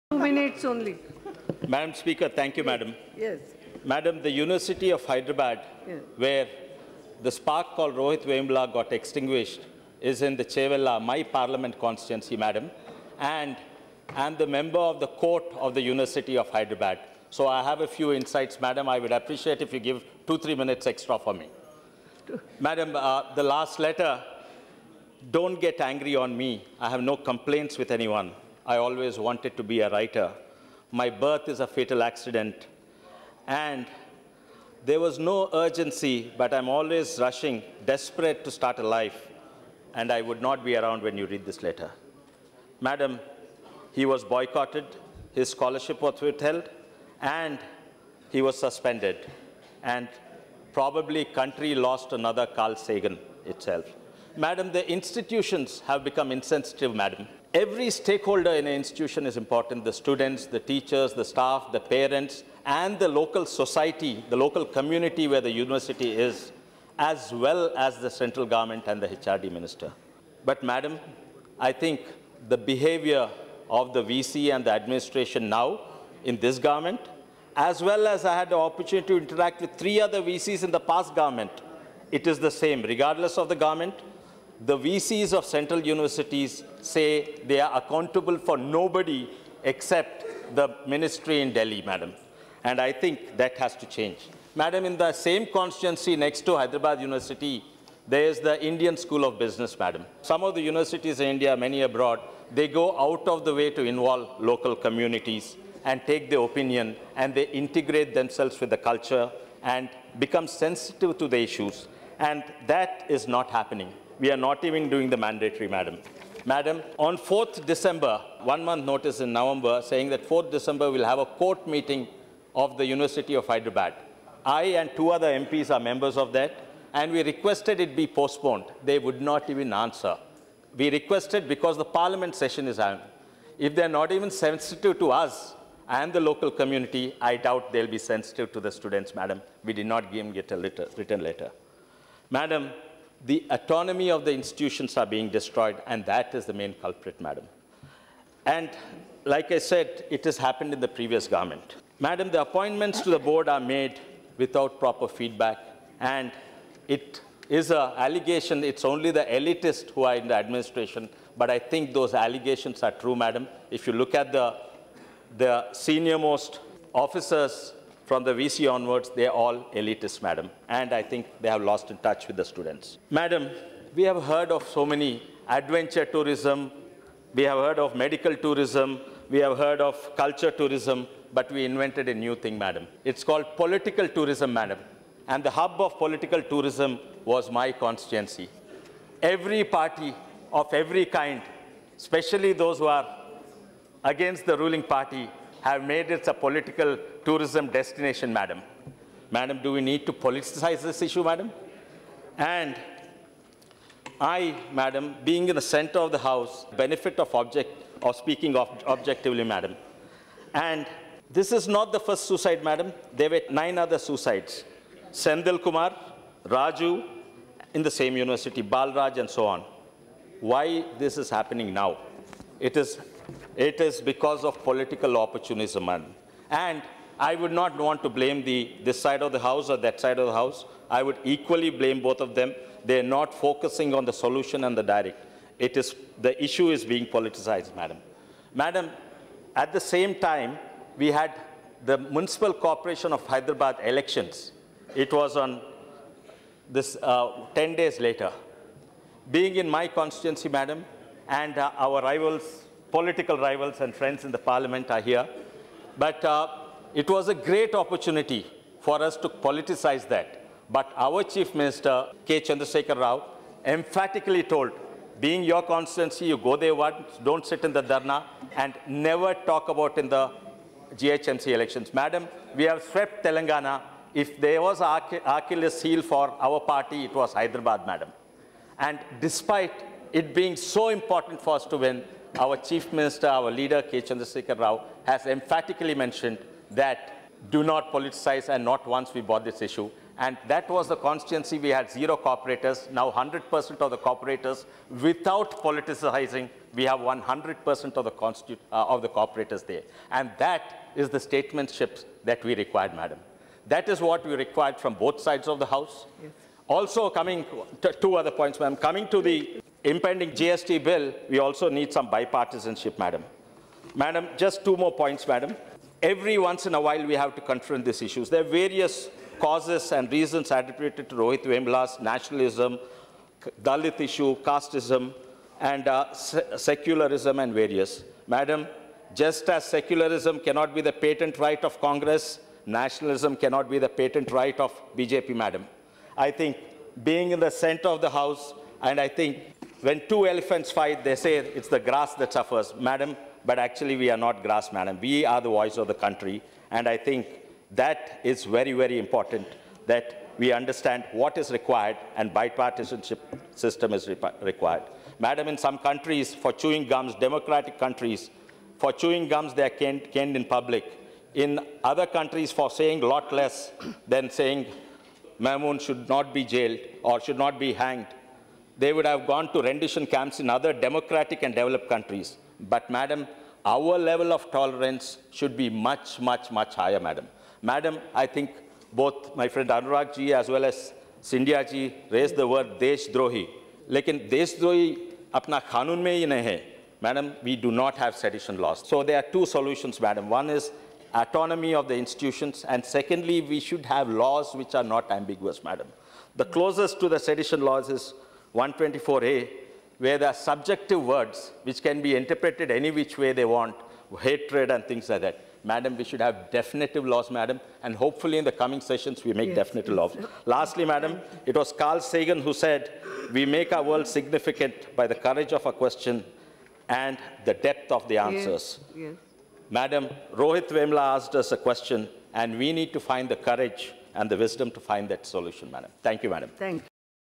LS debate in the Parliament on HCU & JNU
Rohit Vemula's suicide at the Hyderabad Central University HCU was indeed very very sad. The Central Universities should become more sensitive to the students, especially those from socially challenged backgrounds. What is happening in JNU Delhi is undesirable. Instead of finding a solution to these issues, it is unfortunate that the political parties are using it as a political opportunity to score points against one another. I participated in the debate in the Parliament - Lok Sabha. Please watch and give feedback.